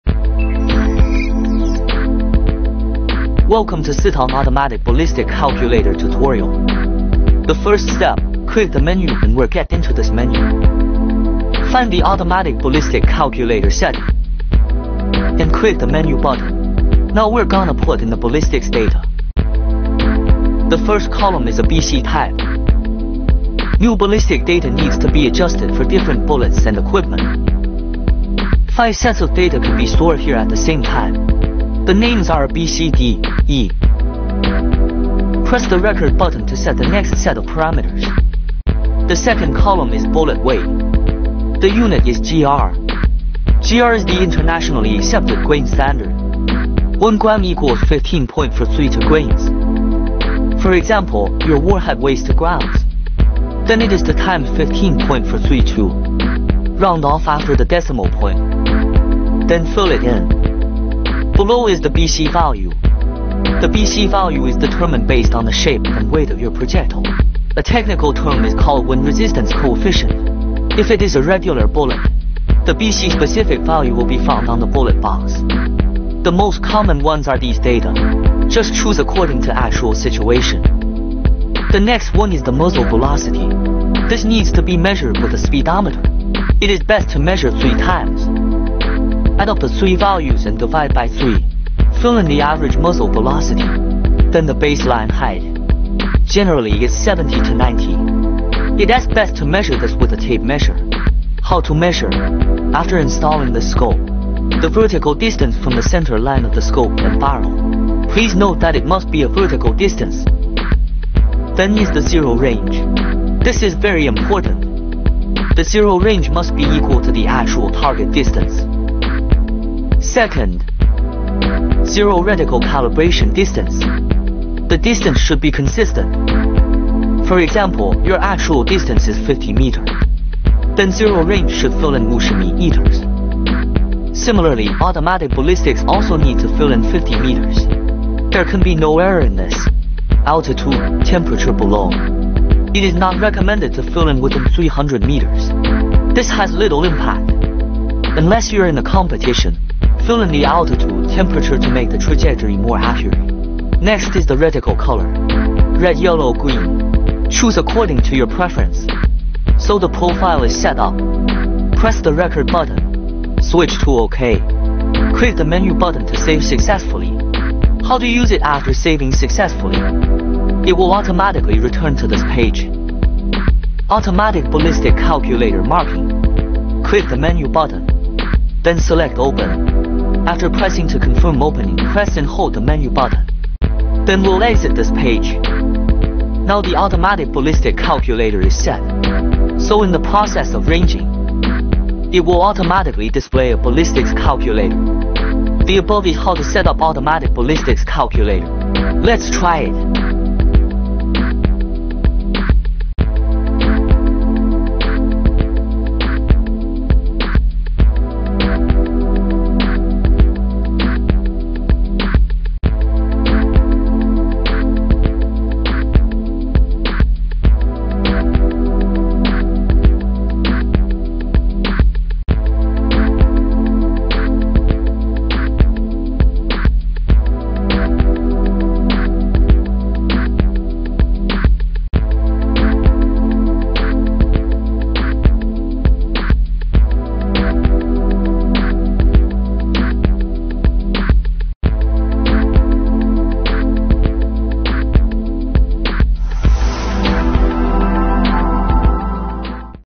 Sytong Auto ballistic calculator guide
All Sytong devices with Auto Ballistic Calculation can learn how to adjust the Auto Ballistic Calculation feature with this video.

I hope this video can help you.